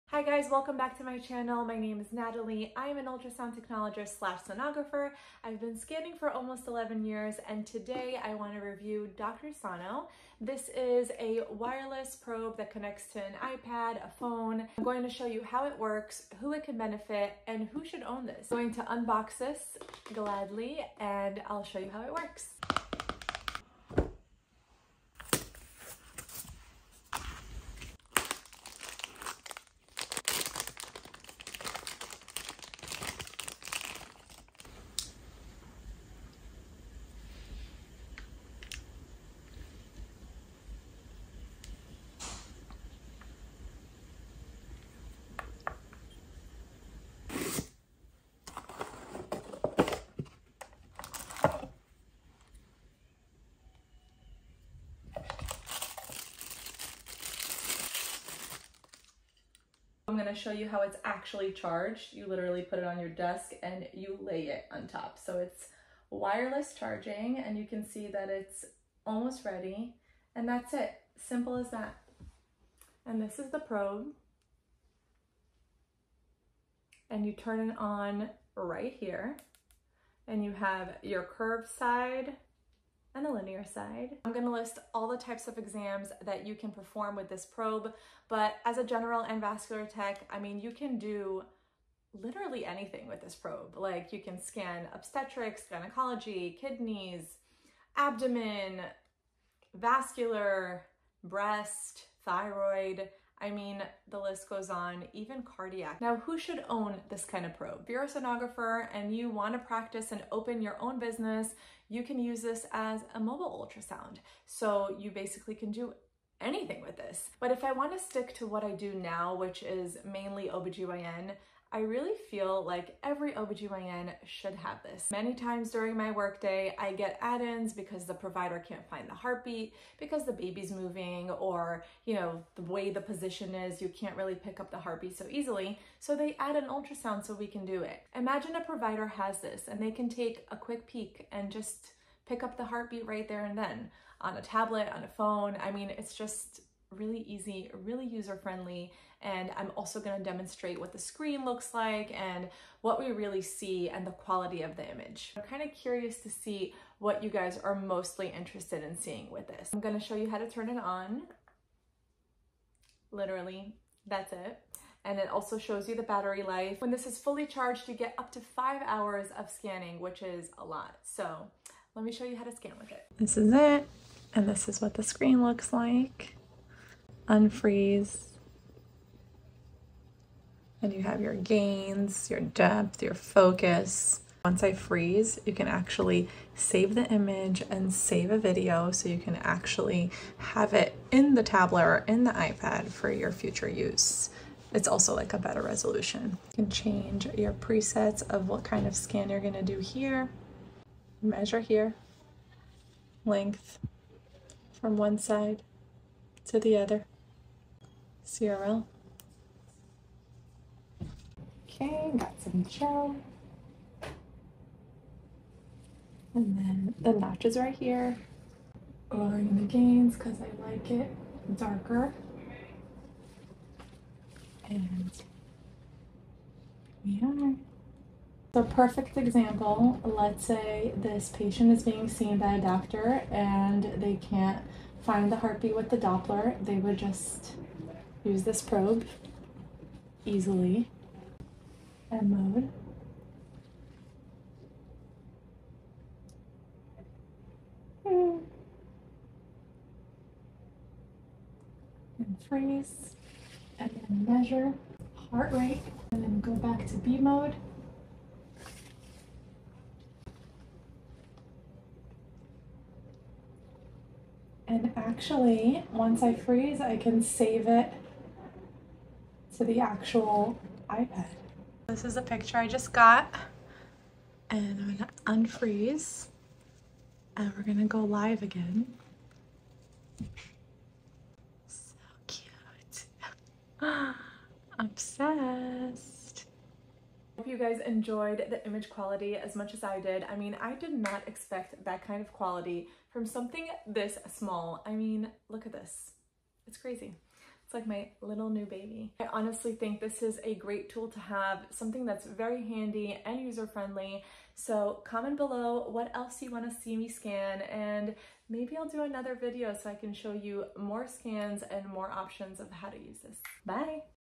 Dr Sono Wireless Ultrasound Probe Review
This model has 3 scan options: linear, curved, or phased, and can be used for full body scans. Choose from Pulsed Wave, Power or Color Doppler or B or M mode-
it is so user friendly and easy to navigate I LOVE IT!

This model has 192 elements for better than HD imaging and no membership fees required.
Windows, Android, and iOS compatible.